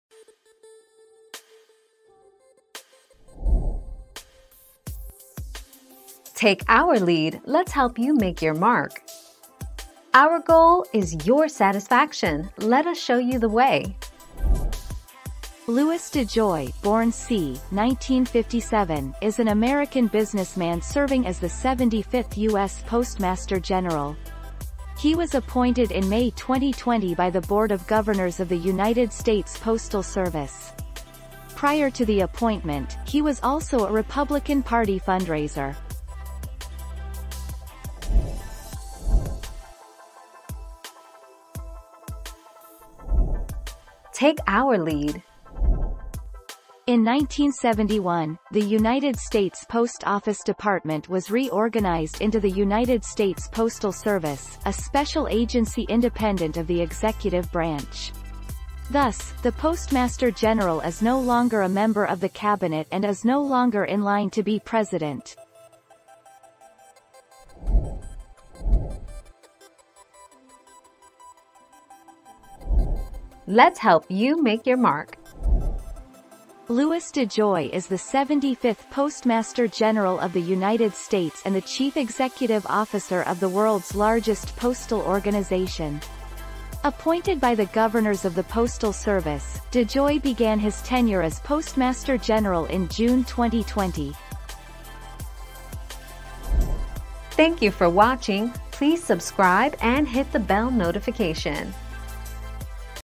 Who is the new postmaster general?
00:00 - Who is the new postmaster general?
00:40 - Is Postmaster General a cabinet position?
01:09 - Who runs the post office?

Laura S. Harris (2021, February 22.) Who is the new postmaster general?
    AskAbout.video/articles/Who-is-the-new-postmaster-general-223986